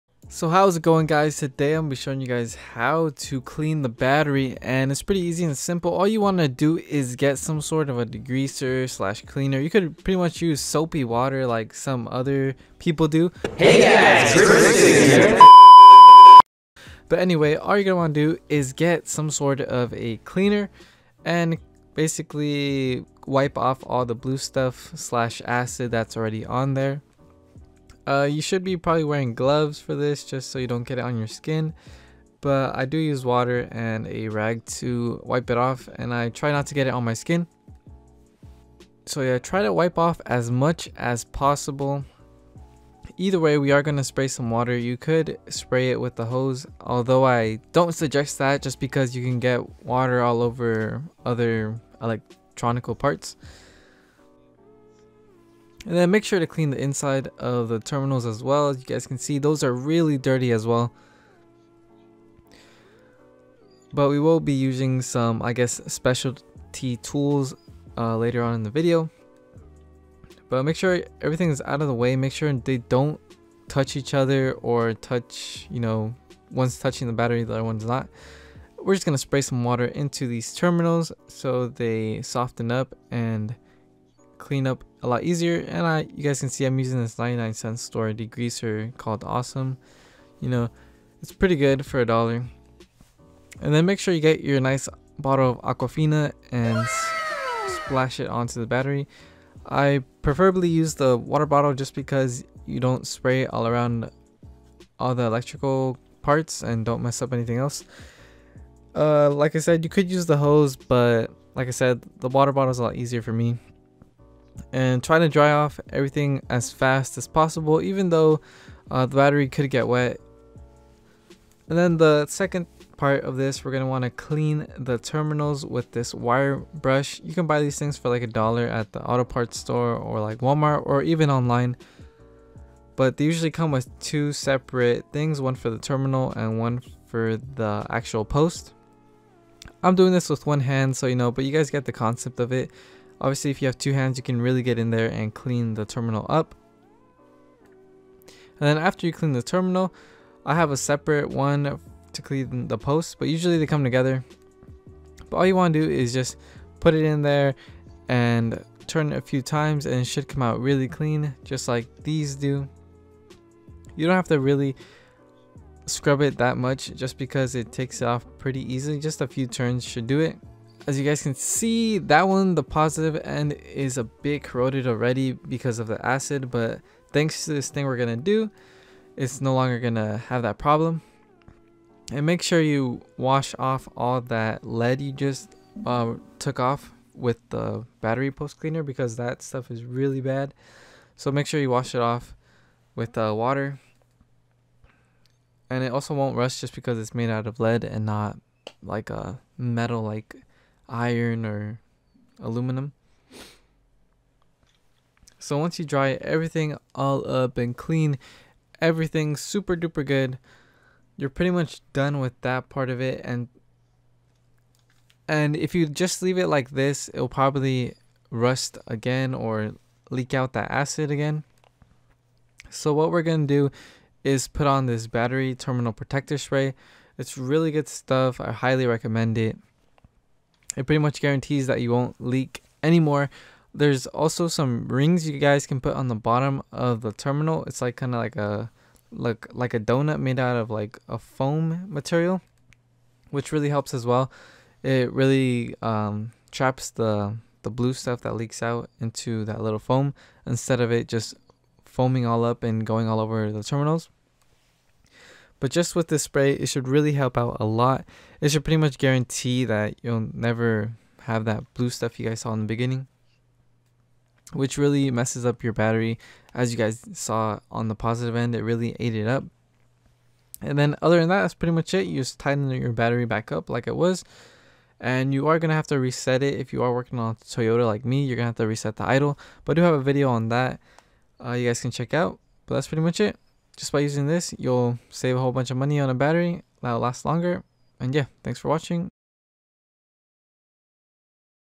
Remove Battery Corrosion FOREVER!
hello today im showing you how to clean the acid off the battery and protect it from future corrosion.